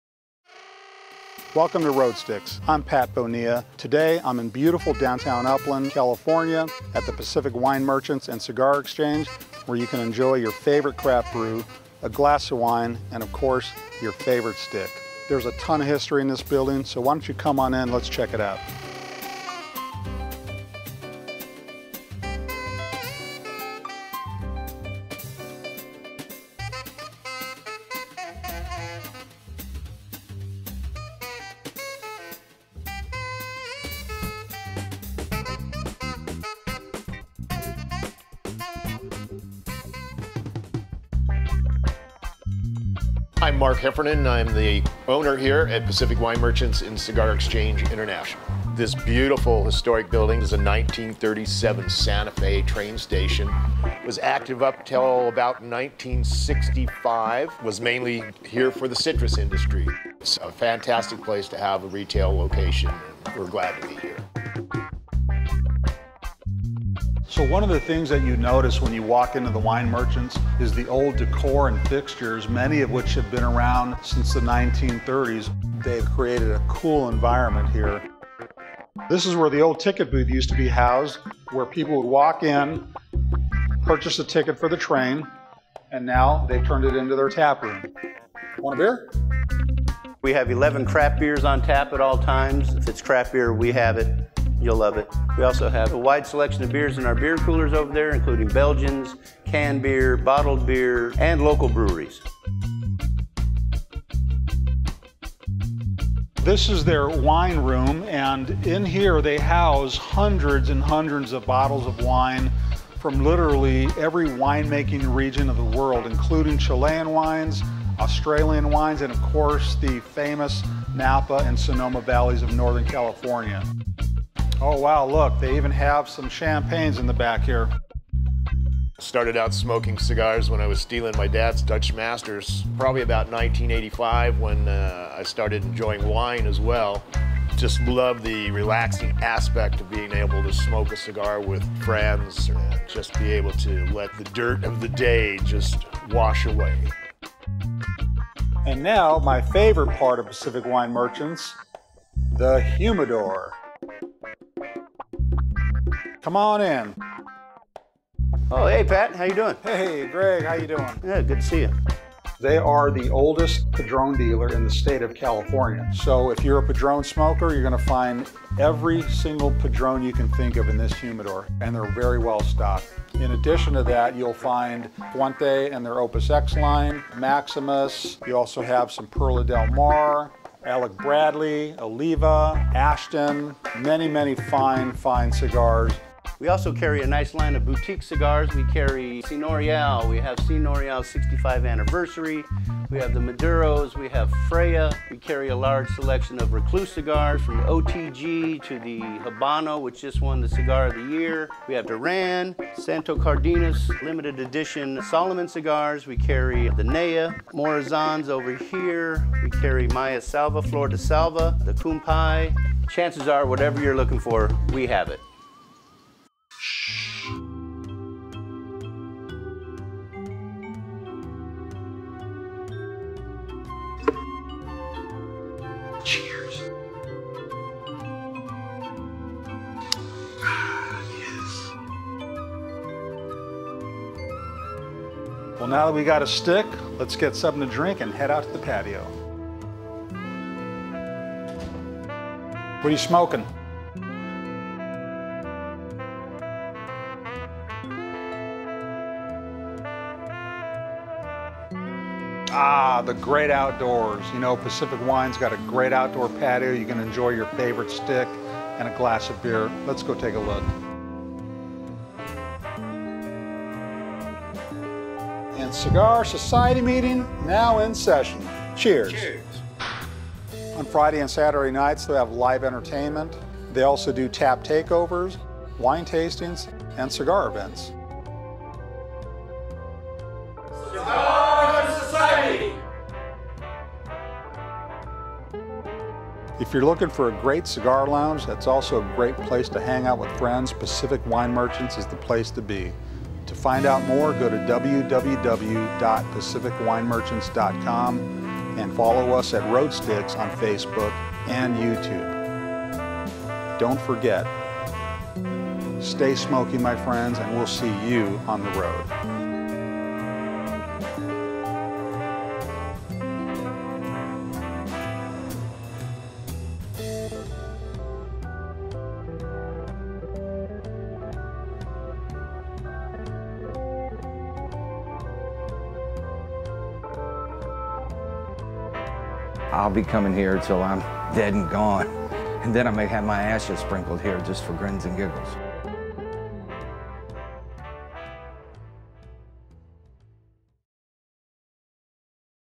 Road Sticks S1 E1 "Pacific Wine Merchants"
Join us as we enjoy some fine sticks at Pacific Wine Merchants in Upland CA.

Stay smokey, and we'll see you on the road!